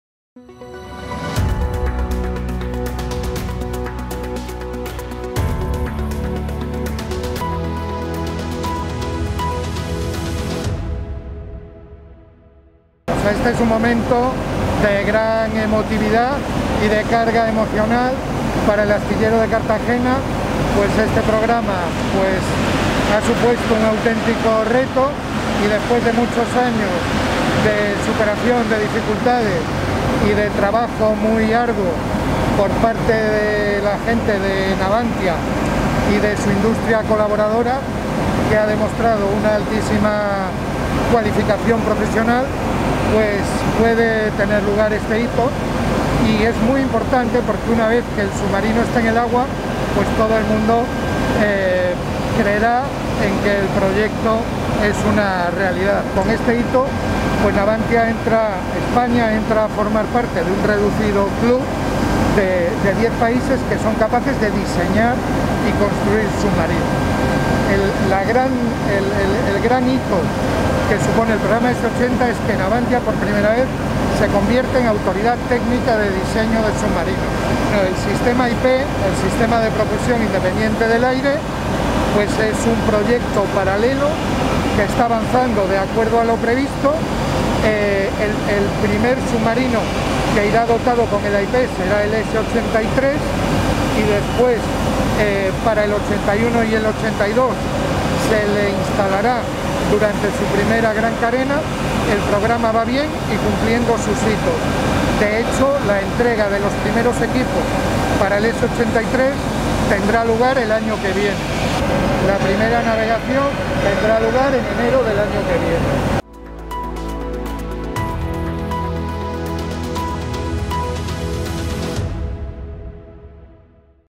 Agustín Álvarez (Navantia): "La primera navegación del S-81 tendrá lugar en enero del año que viene"
El director del Astillero de Cartagena y del Programa S-80 Plus, Agustín Álvarez Blanco, ofrece detalles sobre el programa S-80 Plus. Álvarez dice que este hito significa que "Navantia se convierte en una autoridad técnica para el diseño de submarinos". Además, habla del sistema AIP para submarinos y da una fecha para la navegación del S-81, previsiblemente, en enero de 2022.